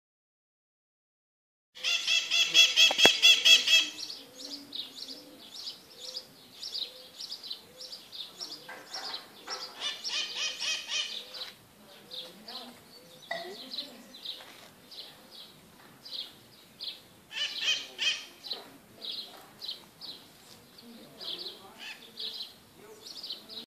Floating spiders! / Aranhas flutuantes!
No, those are not birds. These spiders are crossing a floating web highway!

Não, elas não são pássaros. São aranhas cruzando uma estrada de teias flutuante!